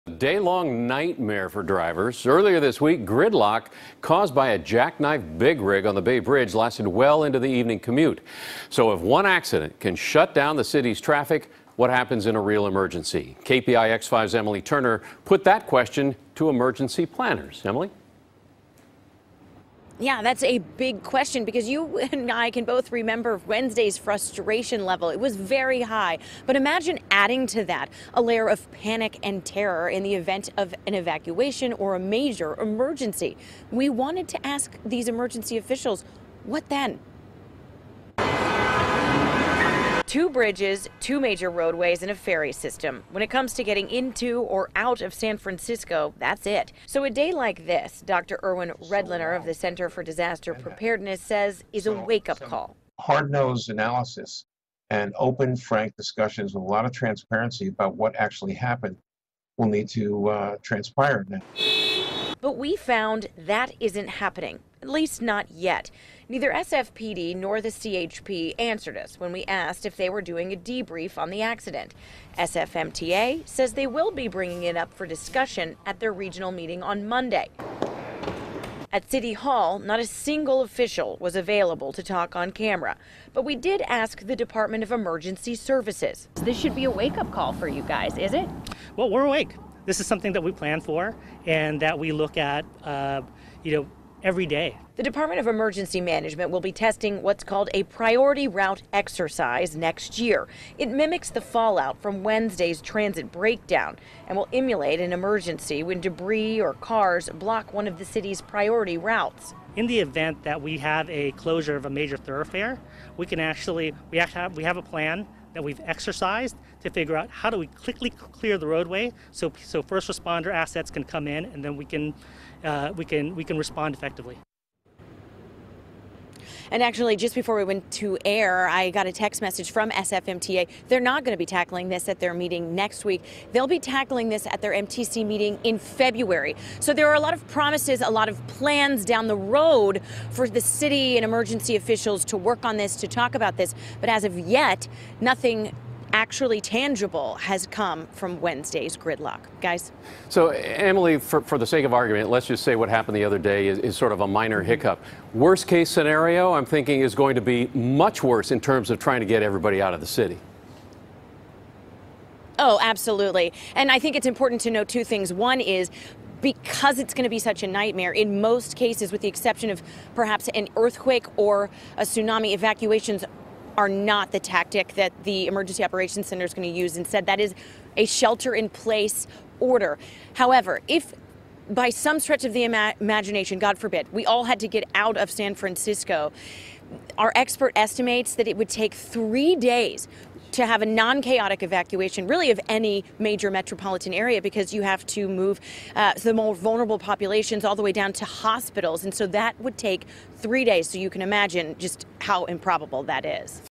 City Emergency Planners Ponder Disaster Evacuations
Wednesday was a nightmare for drivers. Gridlock caused by a jack-knifed big rig on the Bay Bridge lasted well into the evening commute. So if one accident can shut down the city's traffic. So, what happens in a real emergency? Emily Turner posed that question to emergency planners. (12-7-18)